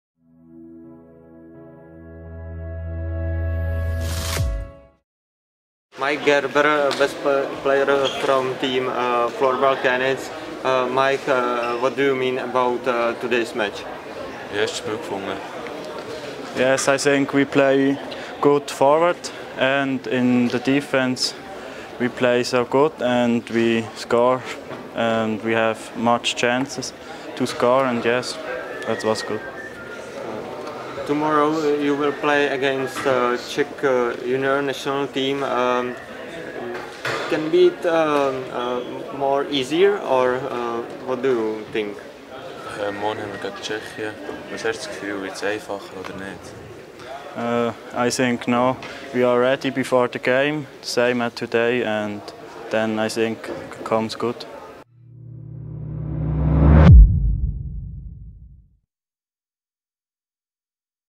ŠKO-ENERGO CUP 2022: Rozhovor s Mikem Gerberem (Floorball Köniz)
Mike Gerber z týmu Floorball Köniz hodnotí utkání na ŠKO-ENERGO CUPU 2022 proti IBK Dalen (9:5).